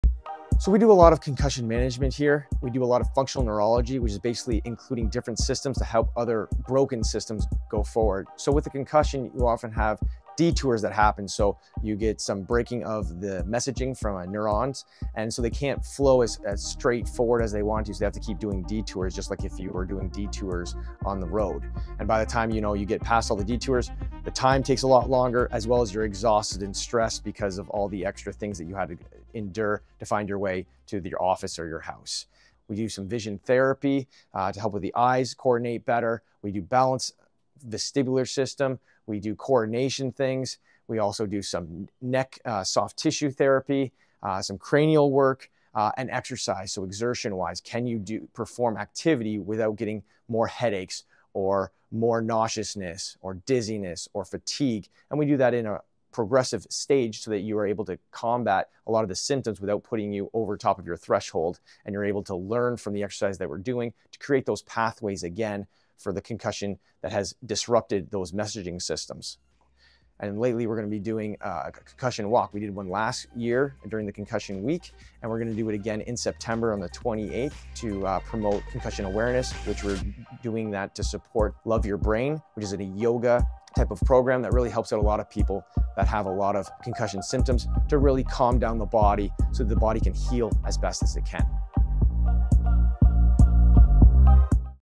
Concussion Explanation and Walk the Block for Concussion Awareness
In this video, we dive into our comprehensive approach to concussion management and functional neurology. When you experience a concussion, your brain's messaging system can get disrupted, leading to detours in communication between neurons. Just like detours on a road, these disruptions can cause delays and additional stress as your brain tries to find alternative pathways.

At our clinic, we focus on helping you rebuild these pathways through a variety of therapies:

Vision Therapy: Enhances eye coordination to improve visual processing.
Balance & Vestibular Training: Improves your balance and reduces dizziness.
Coordination Exercises: Helps you regain smooth and coordinated movements.
Neck Soft Tissue Therapy & Cranial Work: Alleviates neck pain and promotes better cranial function.

Exertion Exercises: We gradually increase your activity levels to help you build tolerance without triggering symptoms like headaches, nausea, dizziness, or fatigue.
Our goal is to help you combat concussion symptoms in a progressive manner, allowing your brain to heal and regain its normal function without pushing you past your threshold.

We're also hosting our Concussion Walk on September 28th as part of Concussion Awareness Week! This event supports "Love Your Brain," a yoga-based program that helps individuals with concussion symptoms calm their bodies and minds to promote healing.

Join us in raising awareness and supporting those affected by concussions. Learn more about our services and how we can help you on your journey to recovery.